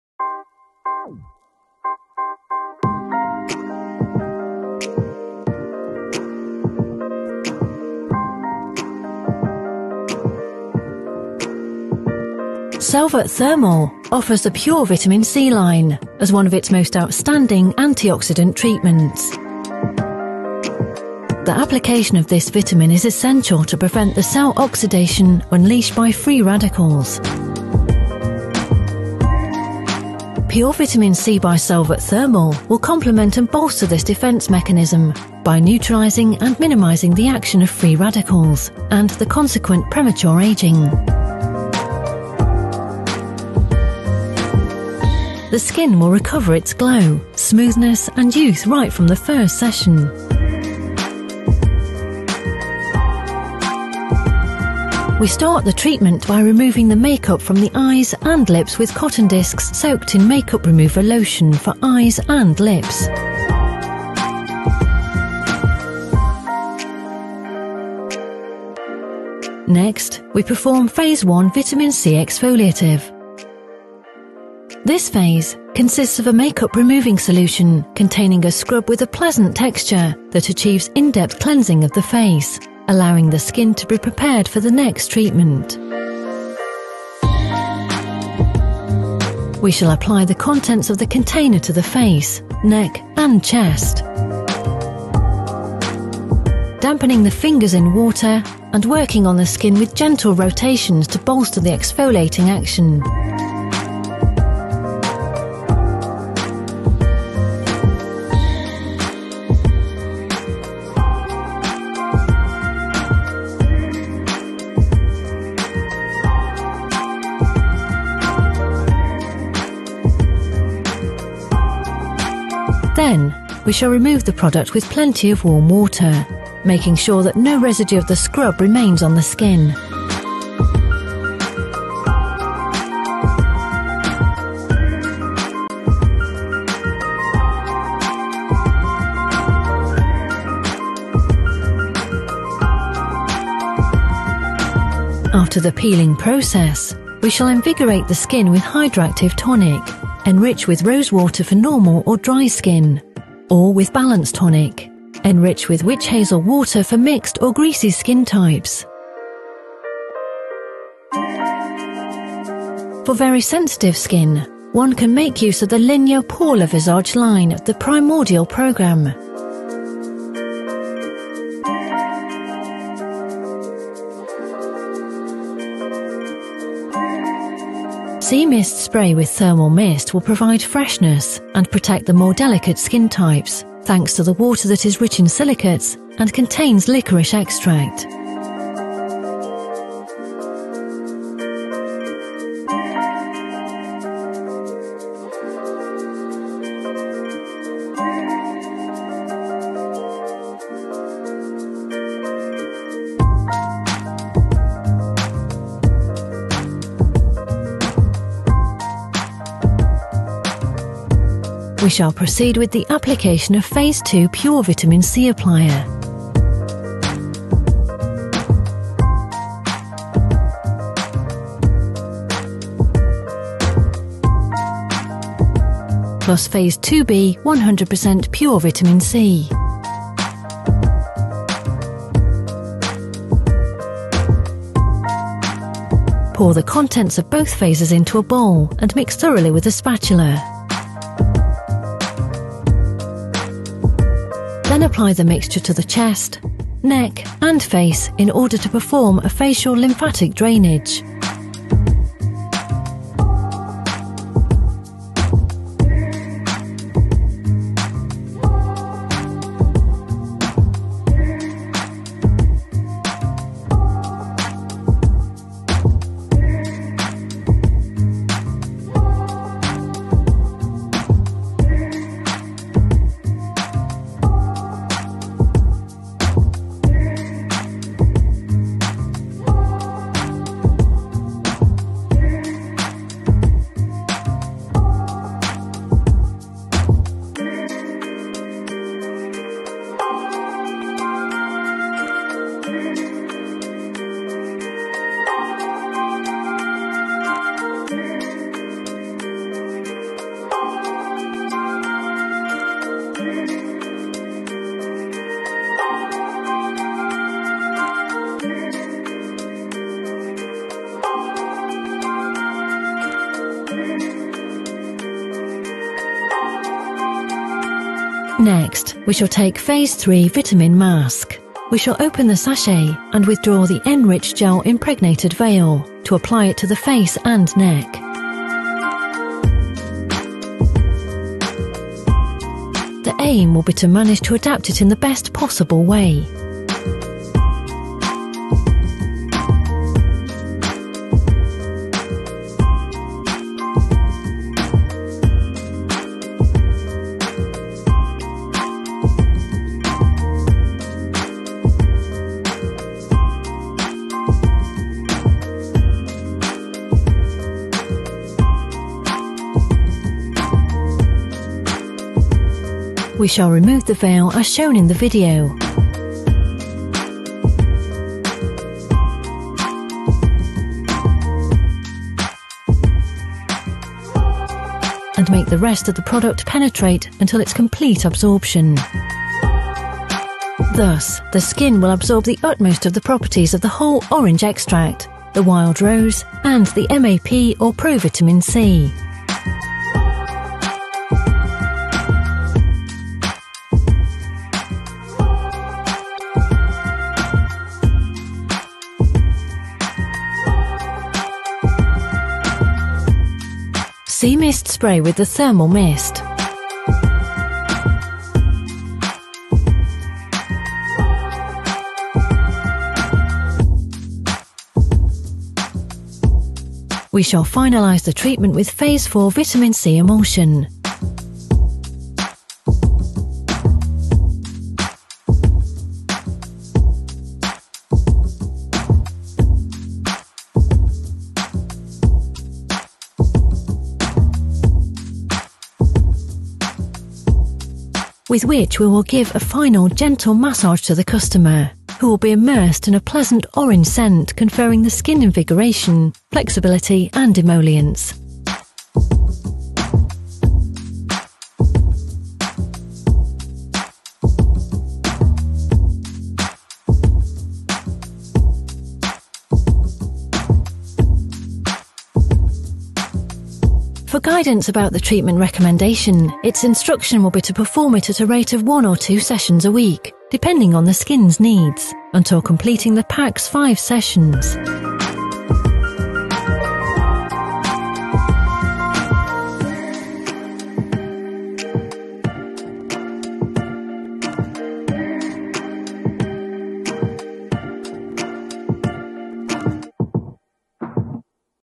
Selvert Thermal Pure VIT C ENGY arckezelés
TISZTA C VITAMIN PLUSZ REVITALIZÁLÓ-ANTI-OXIDÁNS HATÁSÚ ARCKEZELÉS
A Selvert Thermal Tiszta C Vitamin Plusz formulája serkenti és megnöveli
a szervezet külső oxidatív folyamatokkal szembeni hatékony ellenálló képességét.
Minimalizálja a szabad gyökök mennyiségét, a káros oxidatív folyamatok mindenkori aktivitását,
ezáltal megakadályozza a bőr leépülését, a bőr korai öregedését.
A Tiszta C Vitamin Plusz kezelés hatásra a bőr visszanyeri természetes fényét,
üde fiatalos megjelenését, frissességét.

A Selvert Thermal Tiszta C Vitaminos arckezelése azért különleges,
mert az arckezelésben alkalmazott termékek nagy dózisban tartalmaznak
tiszta L-Aszkorbinsavat és Magnézium Ascorbyl Foszfátot (MAP),
melyek koncentráltságuknak köszönhetően rendkívül jó eredményt adnak.